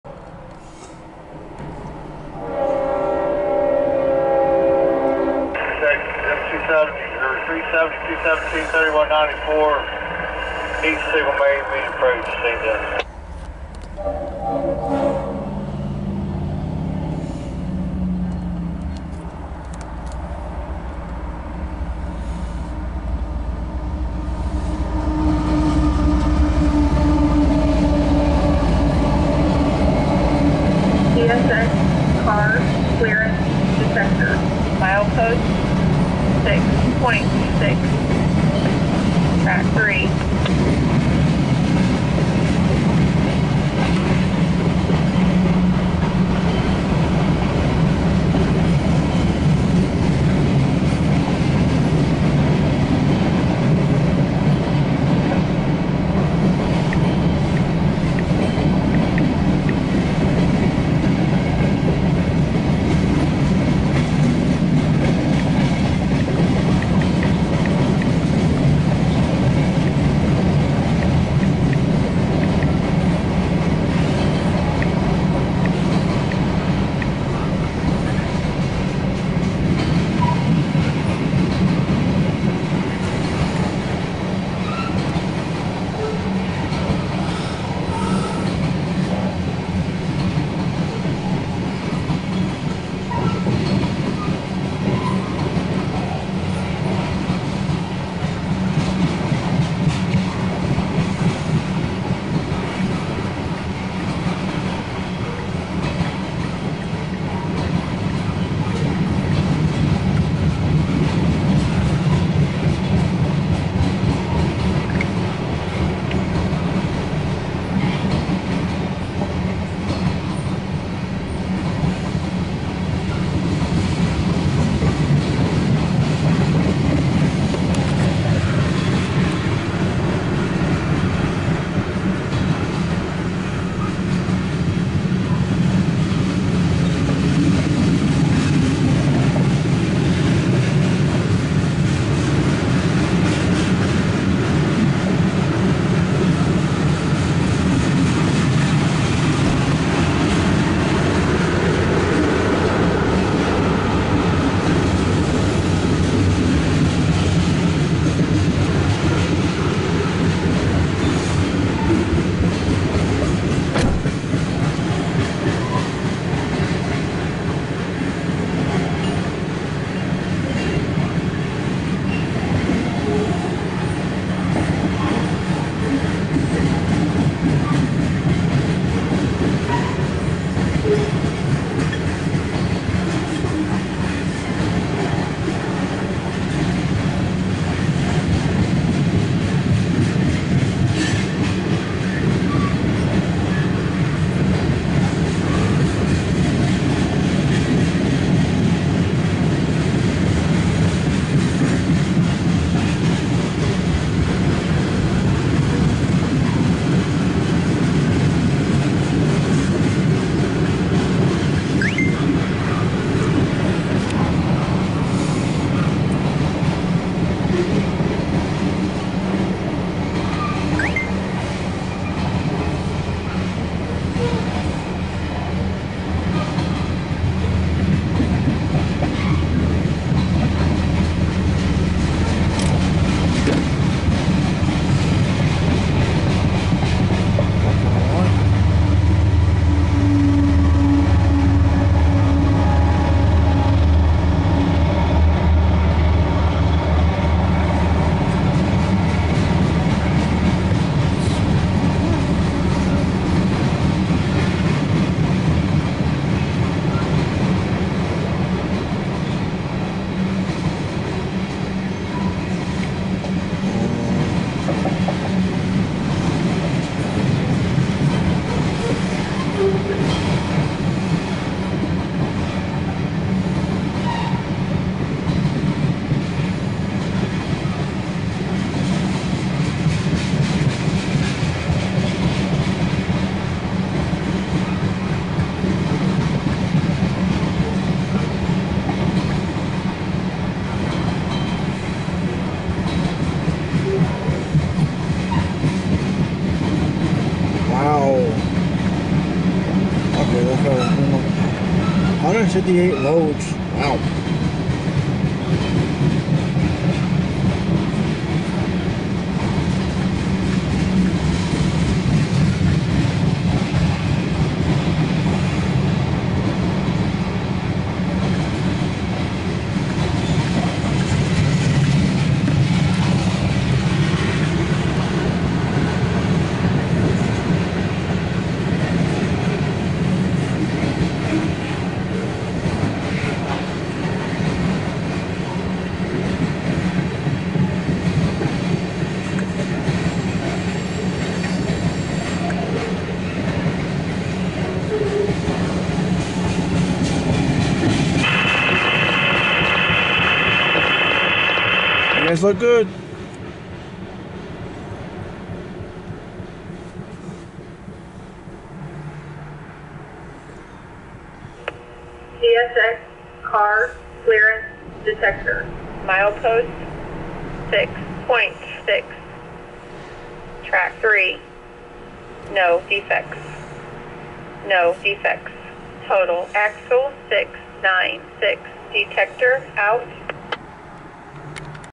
#16889 CSX M372-17 Mix Freight train with 3194
CSX M372-17 Mix freight train is heading eastbound on track 3